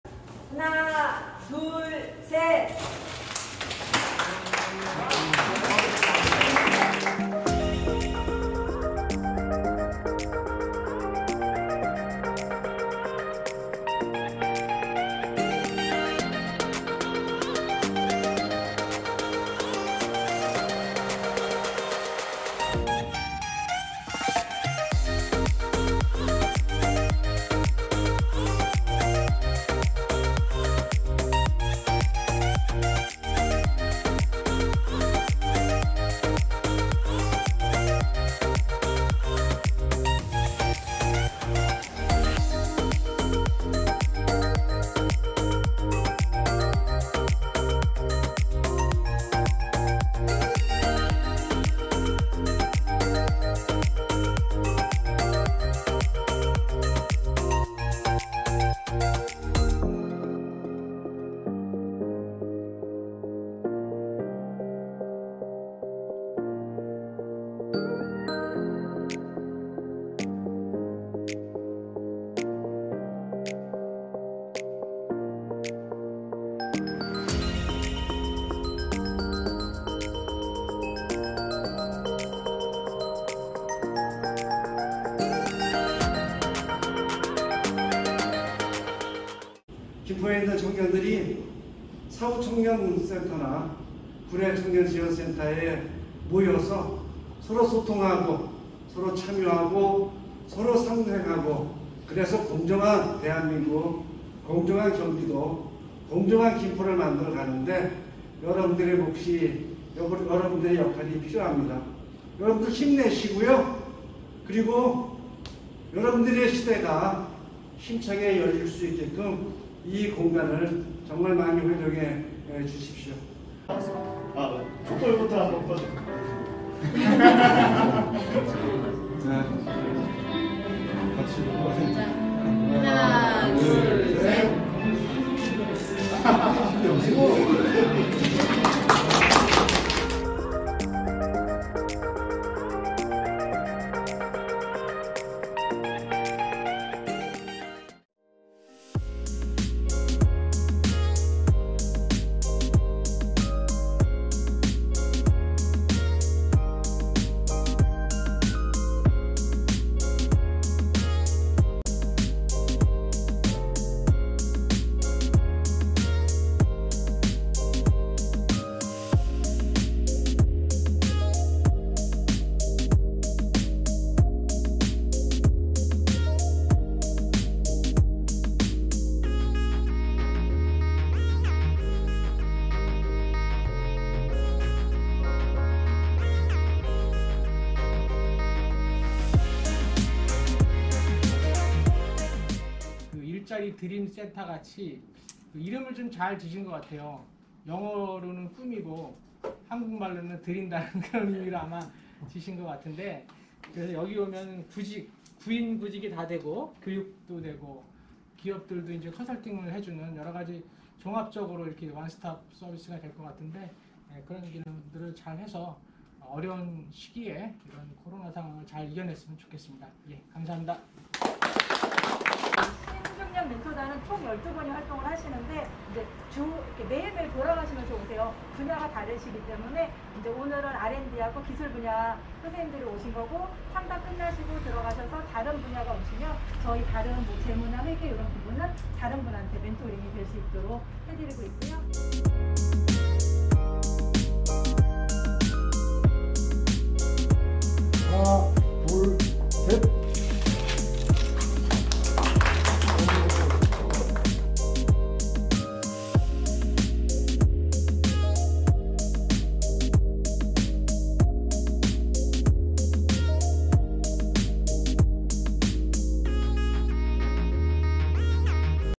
유튜버부터 산단 취직과 기업 컨설팅까지 AtoZ! 구래청년지원센터와 일자리드림센터로 김포 청년 취뽀하자!
유튜브 크리에이터부터 산단 취직, 기업에겐 맞춤 컨설팅까지 AtoZ!

김포창공 구래청년지원센터에서 공간지원/맞춤교육/네트워킹 등
소통과 모임을,
일자리드림센터에서 취업상담, 교육, 기업멘토링을 지원받자!

김포시 김포한강4로 543 8층

김포시 양촌읍 황금로 117 이젠몰 301호

*위 영상은 코로나19 예방수칙을 준수하여 촬영하였습니다*
**위 영상은 김포시청 공보담당관 미디어팀에서 자체제작한 영상입니다**